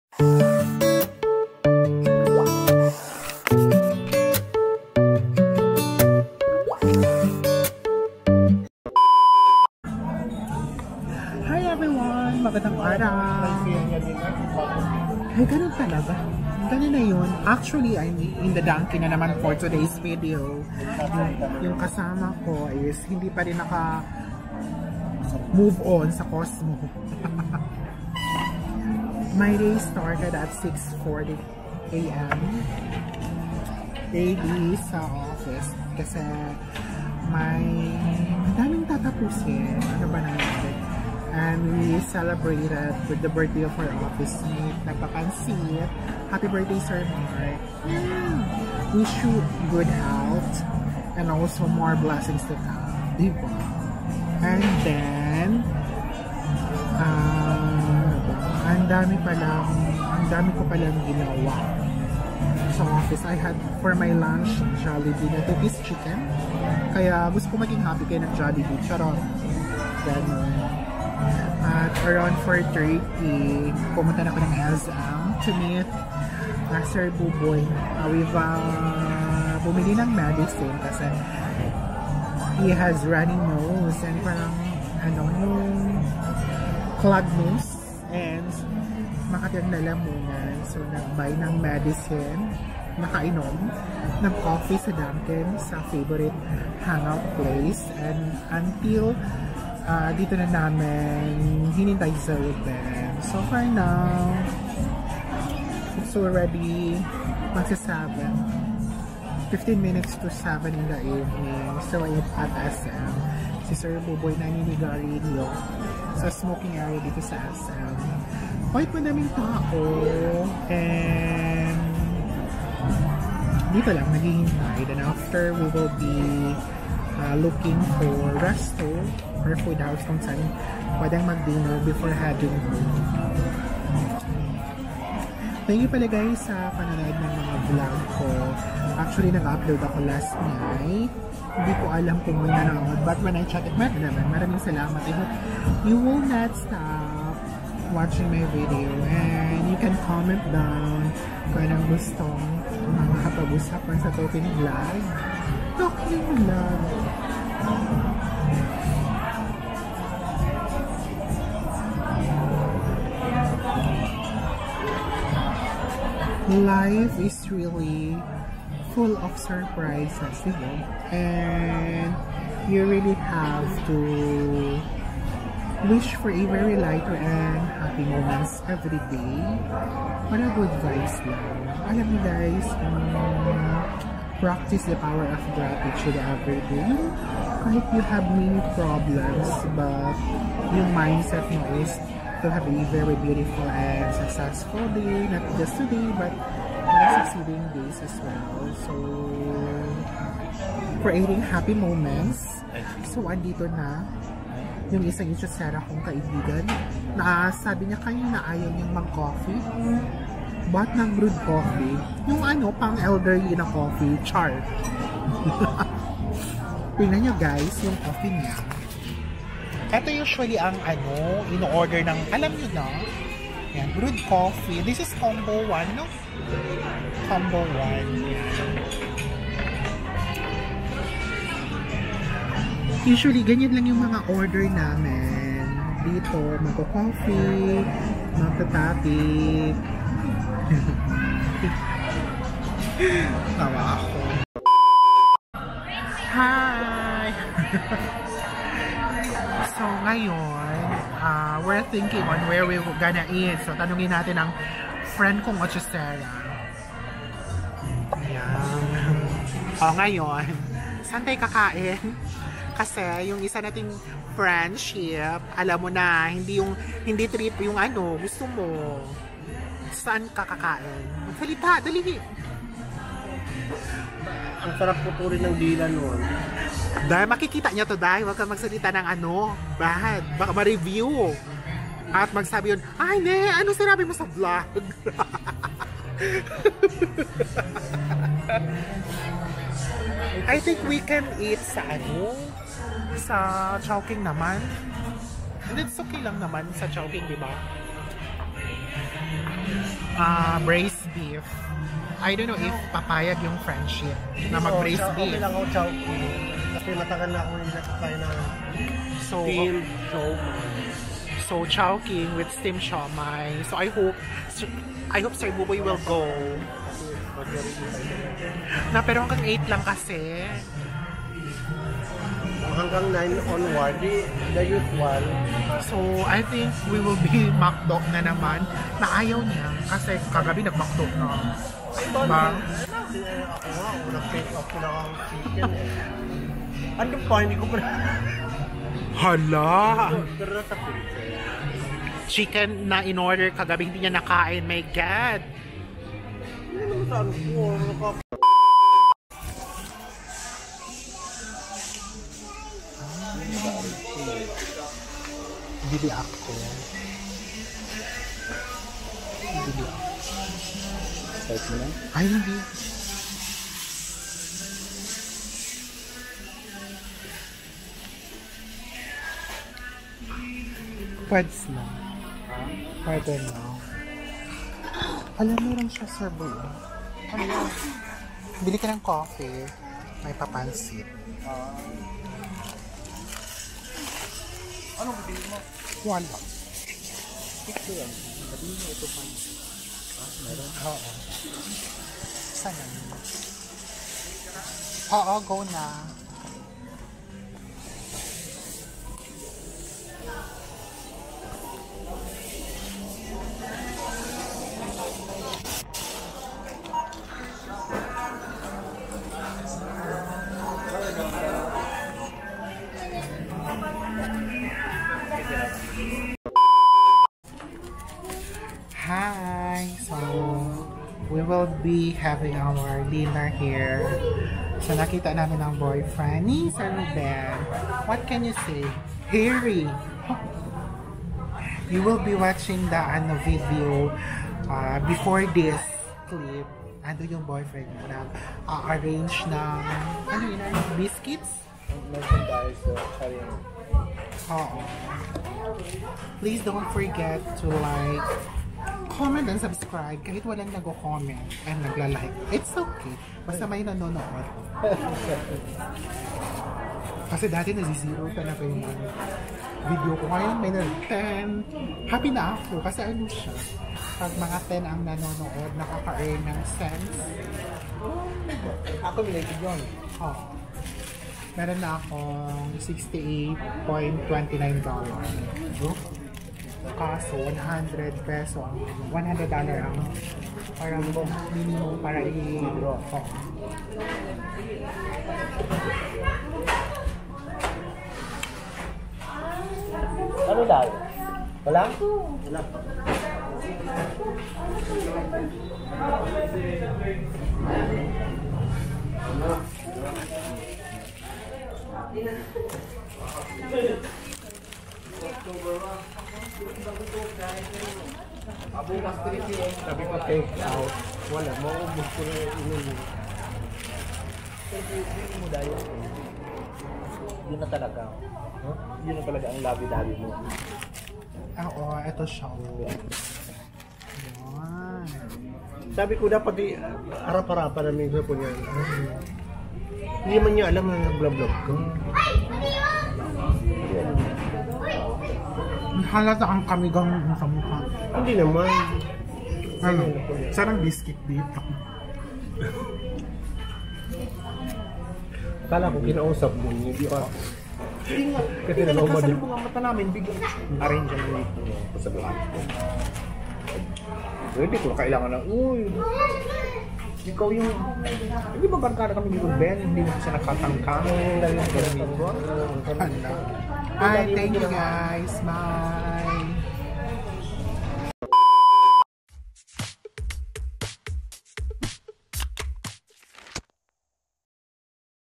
Chill Day with Friends: Iced Coffee at Dunkin, Grocery Run & McDo Dinner + Boyfriend Talk!
Hey guys! ☀️ Come along with me for a super chill day! I grabbed my favorite iced coffee at Dunkin' and spent some time with friends, catching up on everything that happened today. 🧋☕ Sir Buboy picked up some essential groceries at SM Mindpro, and after that, we enjoyed a nice dinner at McDo 🍔🍟. We had a fun conversation about Sir Ruben’s boyfriend too! 😄 Don’t miss the good vibes and simple joys. Hope you enjoy spending the day with us! 💖

Thank you for visiting and watching my humble and simple video.